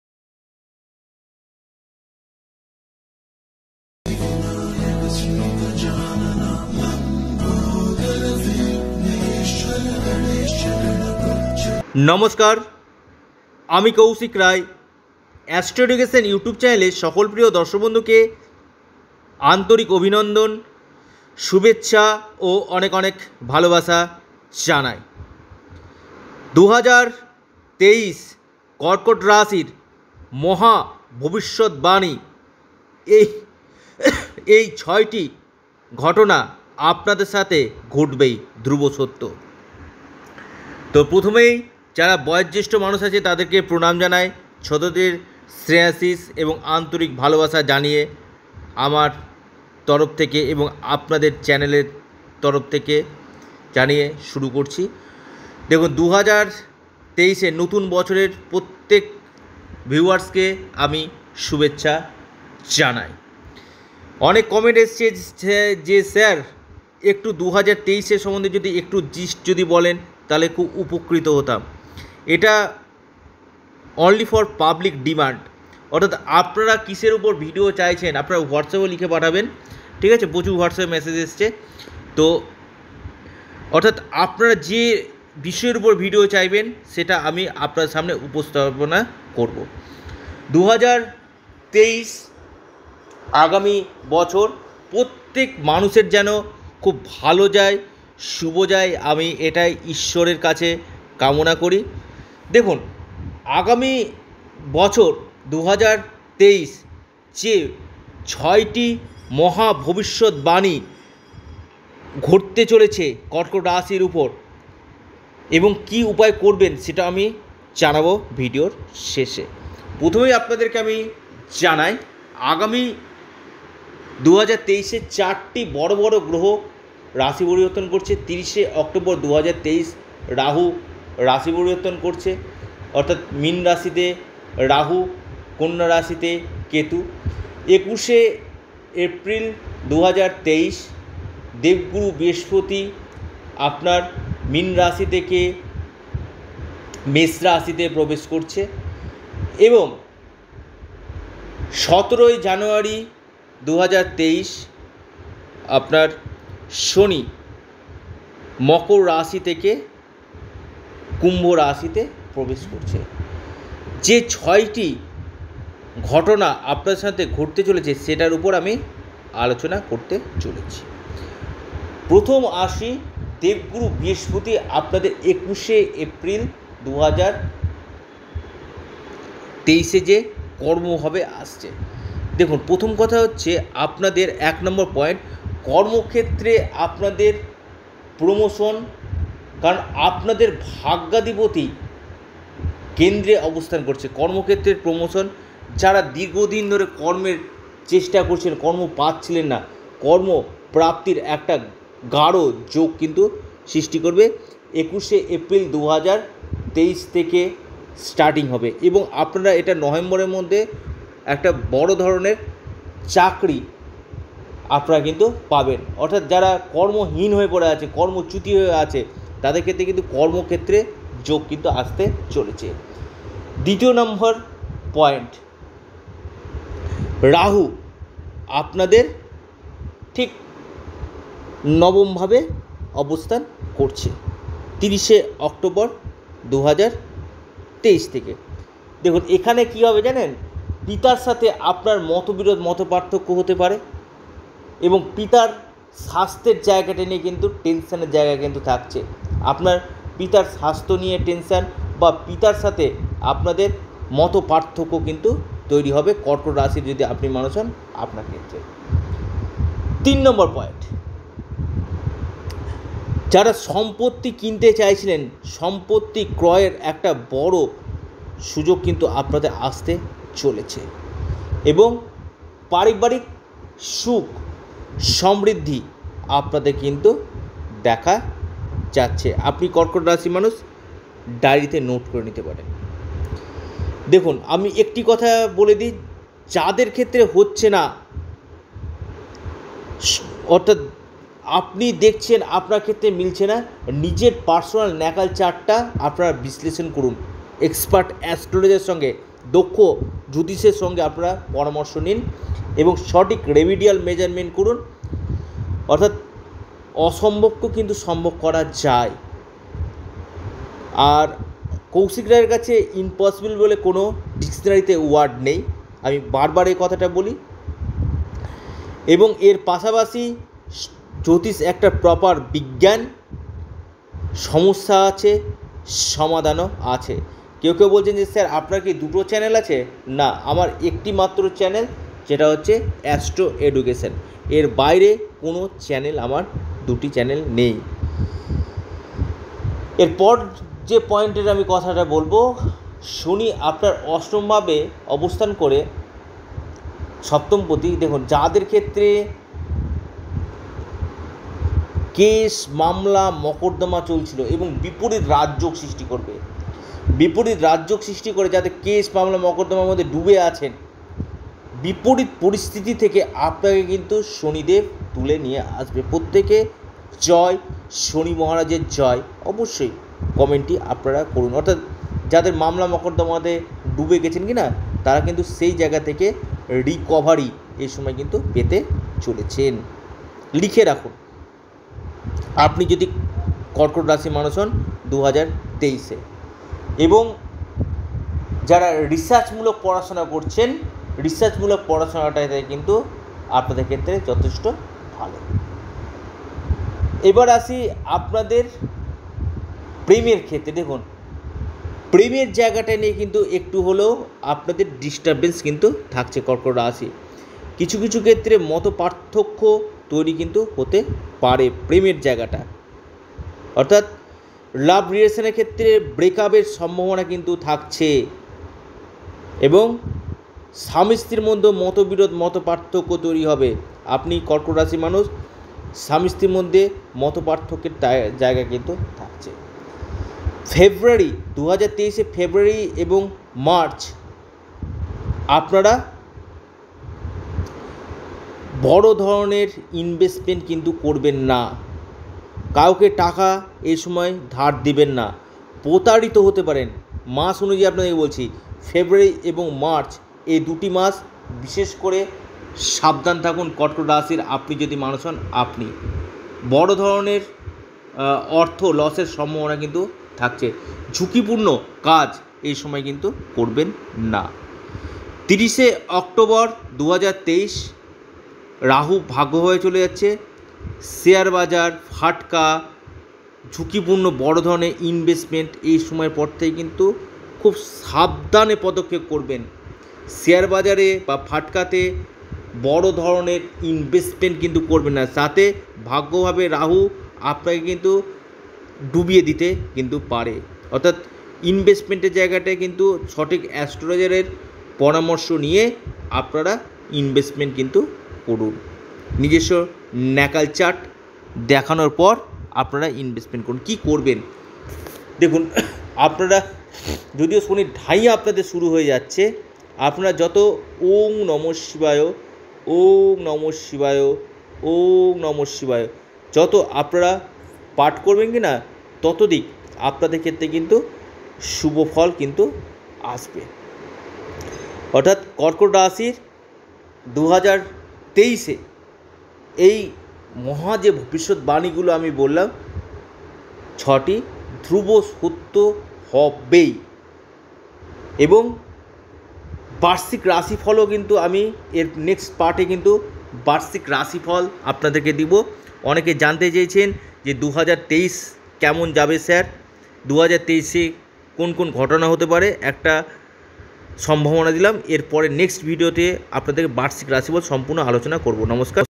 কর্কট রাশি 2023! এই 6টি মহা ভবিষ্যৎবাণী !
কর্কট রাশির মহা ভবিষ্যৎবাণী 2023 এই বড় ঘটনাগুলি আপনার সাথে নতুন বছরে ঘটতে চলেছে।